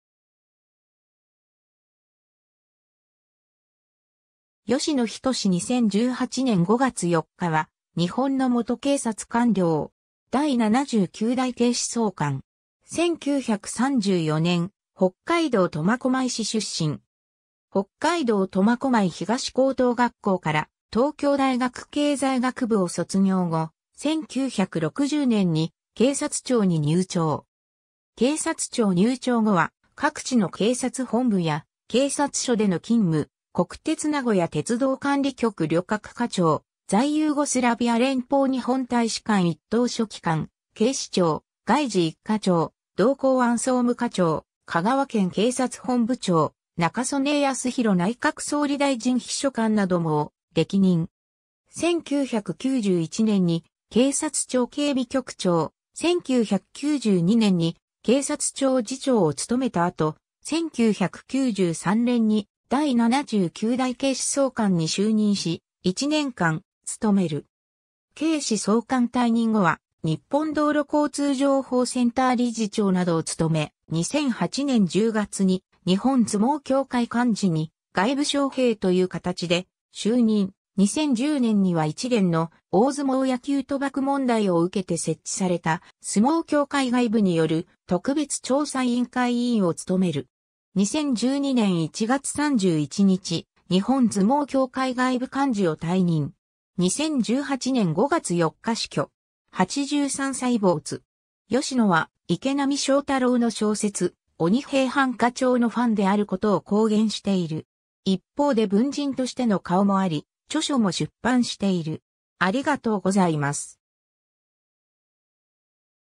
吉野準
吉野 準（よしの じゅん、1934年（昭和9年） - 2018年5月4日）は、日本の元警察官僚。
第79代警視総監。
 1934年、北海道苫小牧市出身。
北海道苫小牧東高等学校から東京大学経済学部を卒業後、1960年に警察庁に入庁。
 警察庁入庁後は各地の警察本部や警察署での勤務、国鉄名古屋鉄道管理局旅客課長、在ユーゴスラビア連邦日本大使館（当時）一等書記官、警視庁外事一課長、同公安総務課長、香川県警察本部長、中曽根康弘内閣総理大臣秘書官などもを歴任。
 1991年に警察庁警備局長、1992年に警察庁次長を務めた後、1993年に第79代警視総監に就任し、1年間務める。
 警視総監退任後は日本道路交通情報センター理事長などを務め、2008年10月に日本相撲協会監事に外部招聘というかたちで就任、2010年には一連の大相撲野球賭博問題を受けて設置された相撲協会外部による特別調査委員会委員をつとめる。
2012年1月31日、日本相撲協会外部監事を退任。
 2018年5月4日死去。
83歳没。
 吉野は池波正太郎の小説『鬼平犯科帳』のファンであることを公言している。
一方で文人としての顔もあり、著書も出版している。